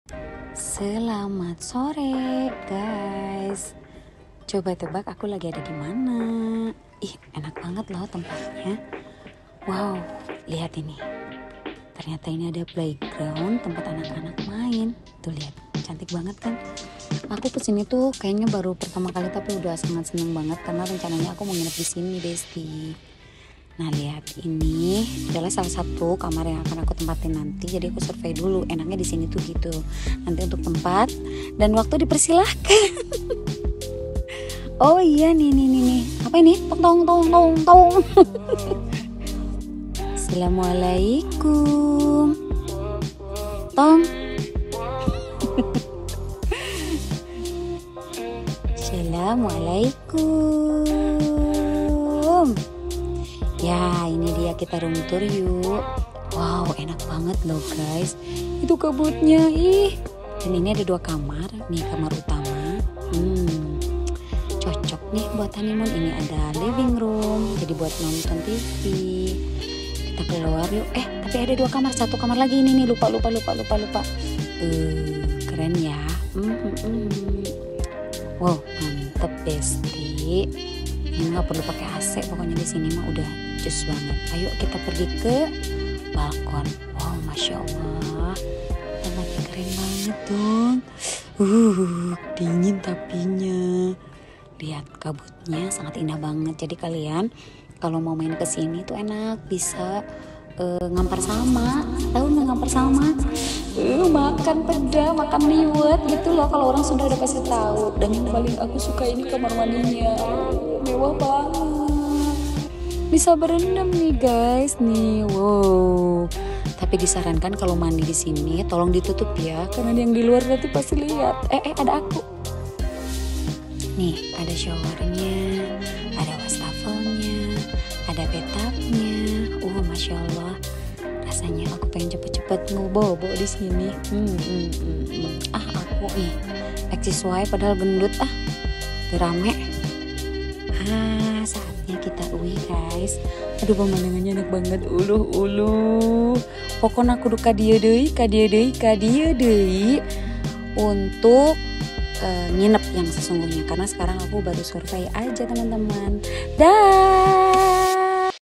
TERBARU⛔ ! Review Glamping Asstro Highlands Ciater Yang Lagi Hits | Glamour Camping by Geulis Gayana
Power by Geulis Gayana
https:lynk.id/geulisgayana